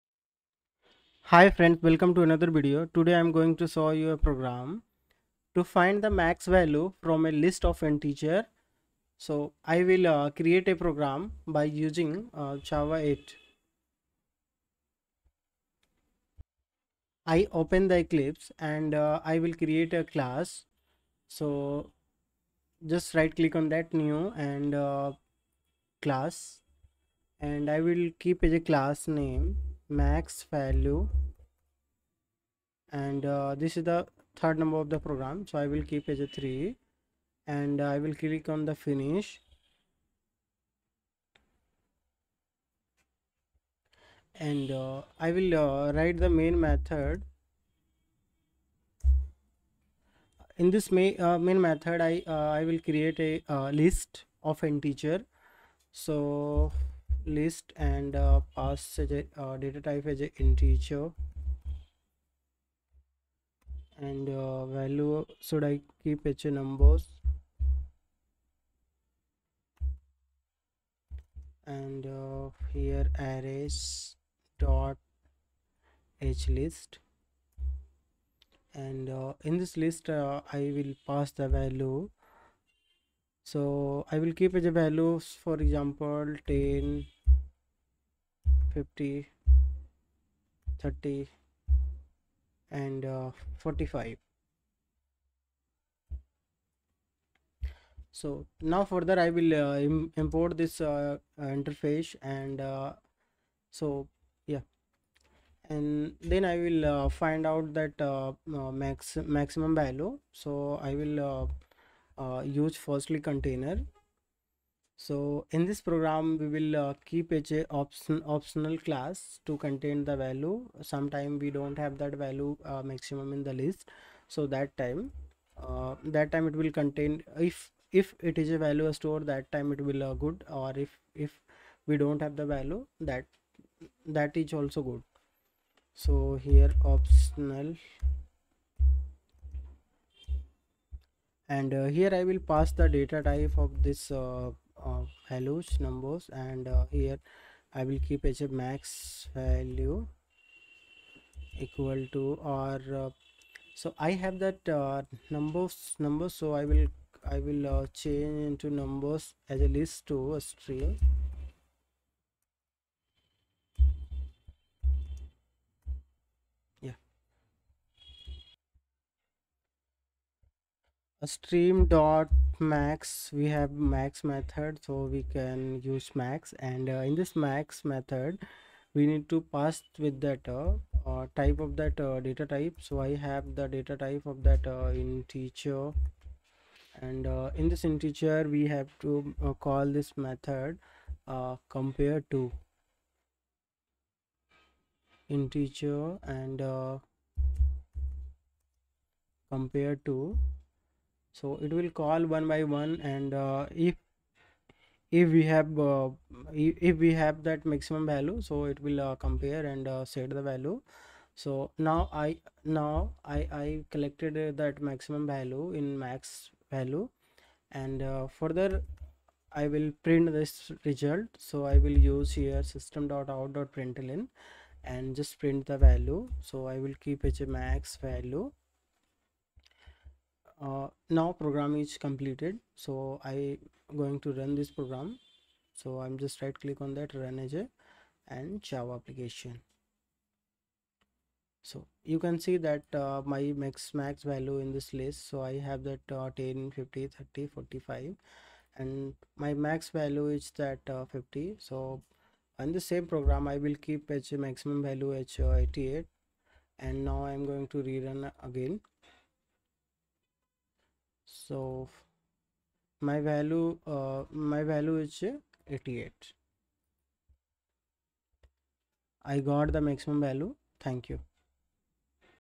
Java 8 Stream API Interview Questions: Find the Maximum Value in List of Integers using Stream API
Q3.  Find the Max Value from a List of Integers using Stream API Java 8

Welcome to Learn Code With Saurabh! In this video,  you will learn: Java program demonstrates how to find the max value from a list of integers using Java 8's Stream API.

Explanation of the code:
The program finds the maximum value in a list of integers using Java Streams.
1. A list of integers (numbers) is created with values like 10,50,30,45,88, etc.
2. The stream() method is called on the list to process the integers as a stream.
3. The max() method is used to find the maximum value, comparing elements using Integer::compareTo.
4. The result is wrapped in an Optional to handle cases where the list might be empty.
5. The if Present() method prints the maximum value if it exists.
Output:
For the given list, the program prints 88, the largest value in the list.